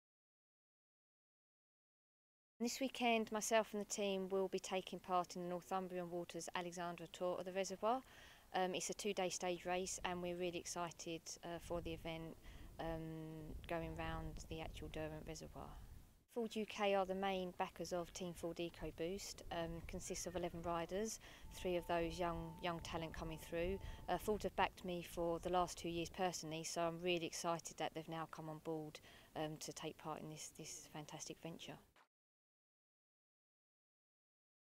Team Ford EcoBoost's Nikki Juniper on Northumbrian Water's Alexandra Tour of the Reservoir
Team Ford EcoBoost's Nikki Juniper on Northumbrian Water's Alexandra Tour of the Reservoir being staged at Derwent Reservoir over the weekend of April 16/17.